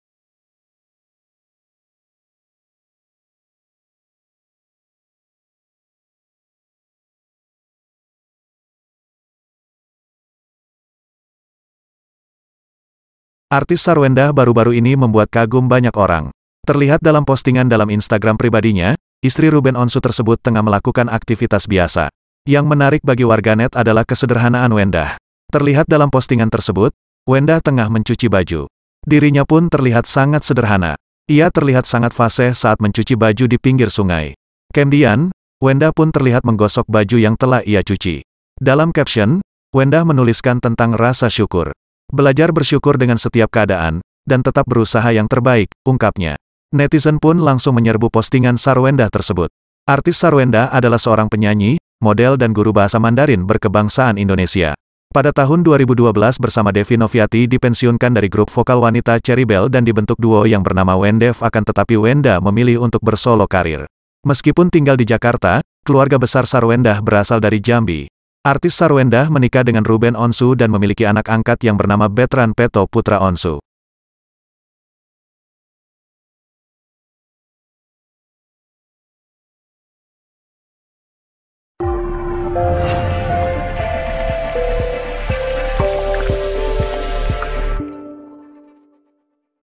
Satu Sikap Sarwendah Buat Kagum Banyak Orang, Rela Lakukan ini Sendiri
TRIBUNJAMBI.COM-Artis Sarwendah baru-baru ini membuat kagum banyak orang. Terlihat dalam postingan dalam instagram pribadinya, istri Ruben Onsu tersebut tengah melakukan aktifitas biasa.

Yang menarik bagi warganet adalah kesederhanaan Wendah. Terlihat dalam postingan tersebut, Wendah tengah mencuci baju.

Dirinya pun terlihat sangat sederhana. Ia terlihat sangat faseh saat mencuci baju di pinggir sungai.(ste)